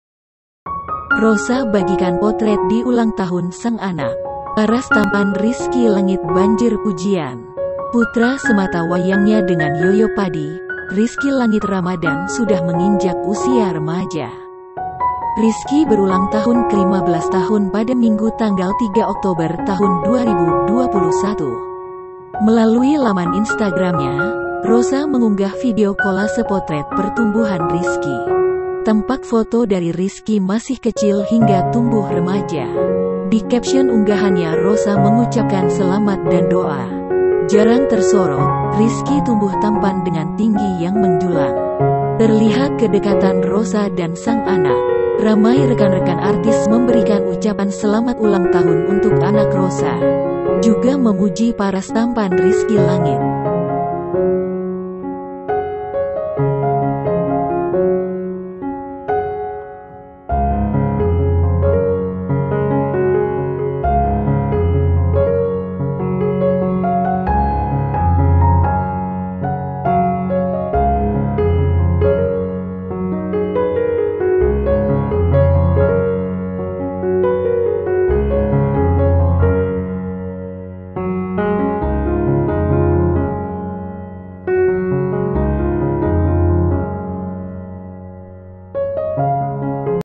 Artis Rossa Pamer Potret Rizky Langit, Wajah Tampan Curi Perhatian
TONTON VIDEO LAINNYA @TribunLampungOfficial
LAMPUNG.TRIBUNNEWS.COM - Rossa bagikan potret di ulang tahun sang anak. Paras tampan Rizky Langit banjir pujian.
Putra semata wayangnya dengan Yoyo Padi, Rizky Langit Ramadhan sudah menginjak usia remaja.
Rizky berulang tahun ke-15 tahun pada Minggu 3 Oktober 2021.
Melalui laman Instagramnya, Rossa mengunggah video kolase potret pertumbuhan Rizky.
Tampak foto dari Rizky masih kecil hingga tumbuh remaja.
Di caption unggahannya Rossa mengucapkan selamat dan doa.
Jarang tersorot, Rizky tumbuh tampan dengan tinggi yang menjulang.
Terlihat kedekatan Rossa dan sang anak.
Ramai rekan-rekan artis memberikan ucapan selamat ulang tahun untuk anak Rossa.
Juga memuji paras tampan Rizky Langit.